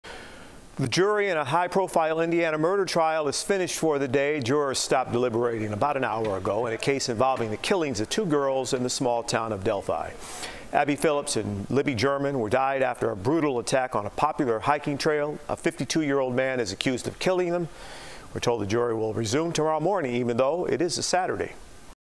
No verdict yet in Delphi double murder trial
The first full day of jury deliberations ended without a verdict on Friday in the trial of Richard Allen, who is accused in the 2017 killings of two teenage girls who had vanished during a hike in Delphi, Indiana.